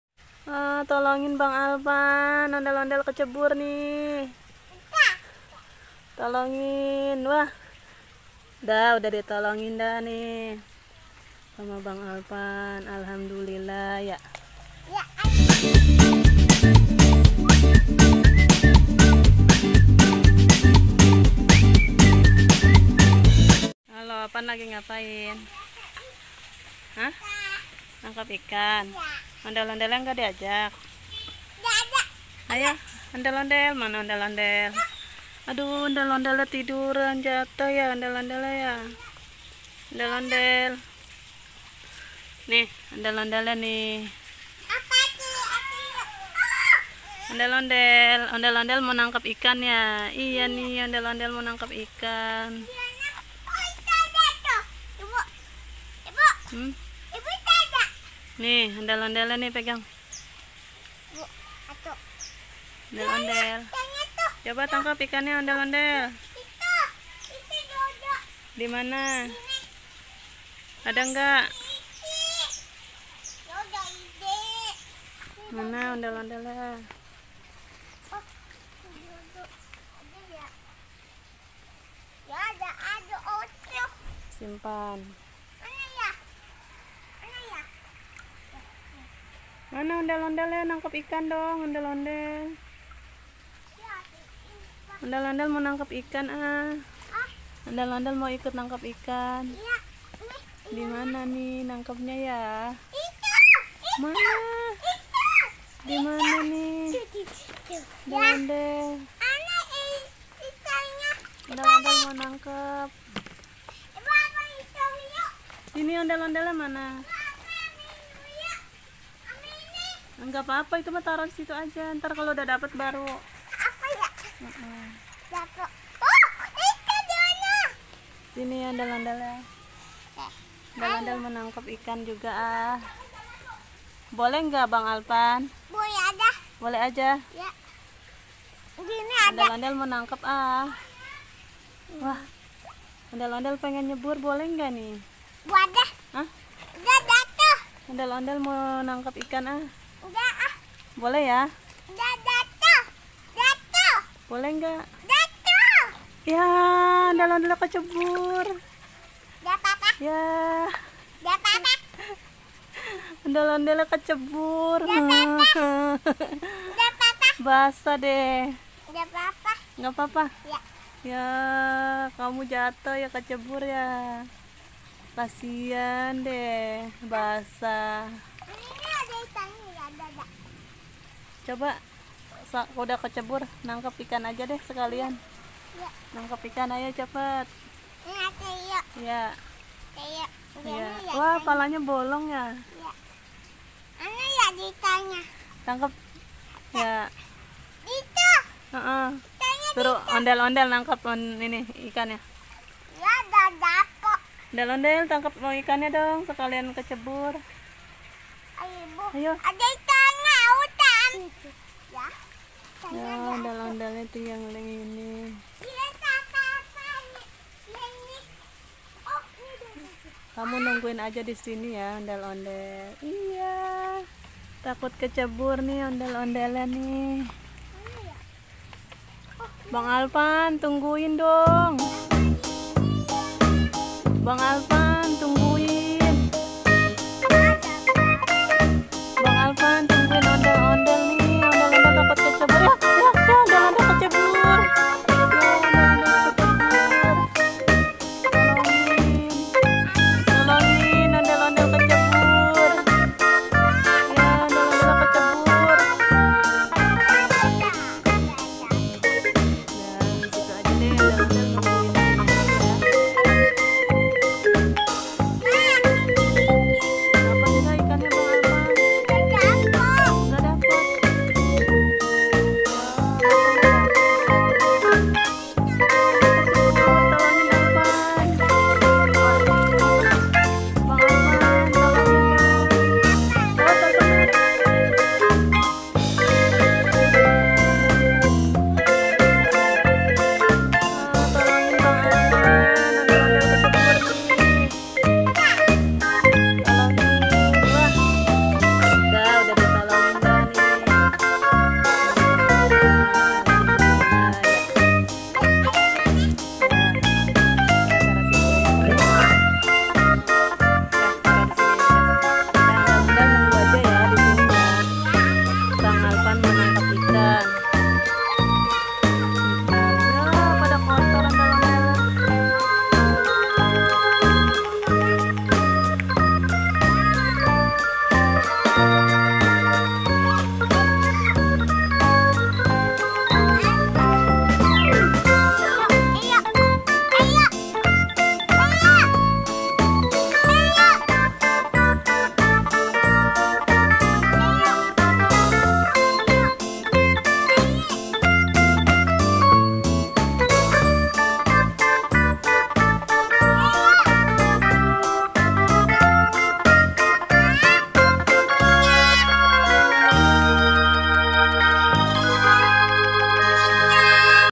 Ondel Ondel Lucu | Ondel Ondel Mini Kecebur di Kolam Ikan
Hai alvan lagi main sama Ondel Ondel lucu yang kecebur di kolam ikan sampai basah yuk ikuti terus video lainnya ya terimakasih